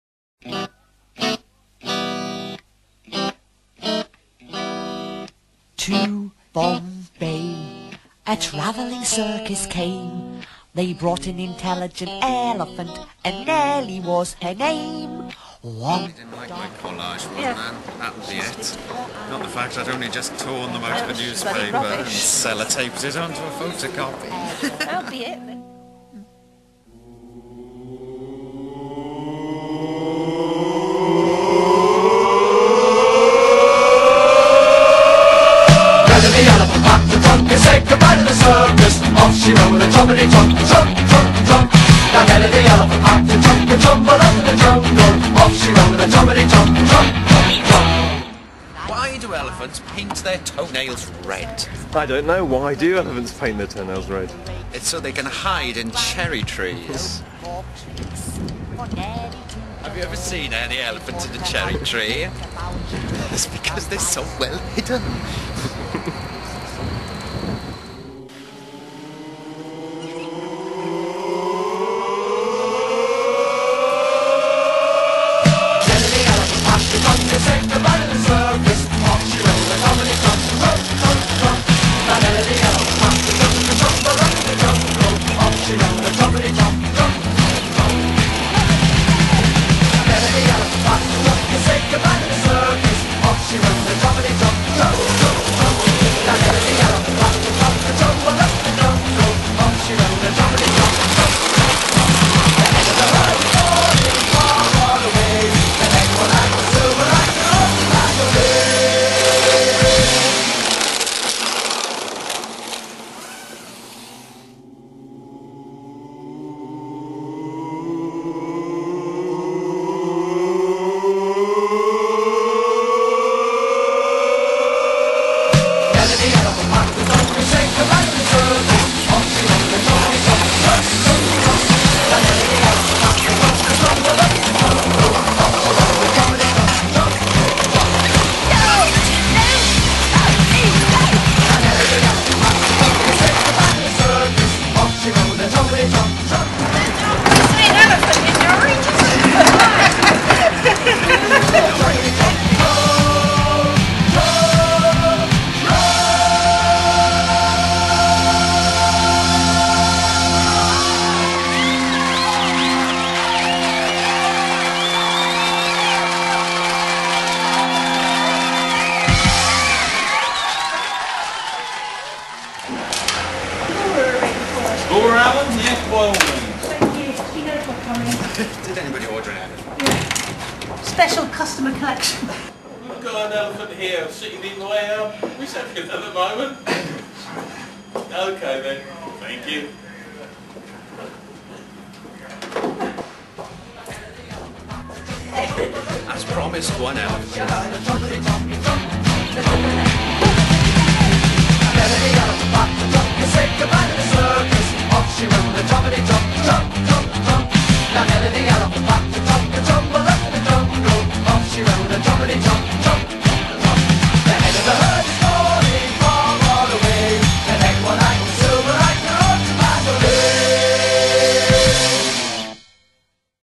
Go Elephants!
Tim Edwards and Fiona Muller take a large orange fibreglass baby elephant across the city of Norwich.

Contains one very corny joke, an excited Norfolk granny and a bewildered security guard.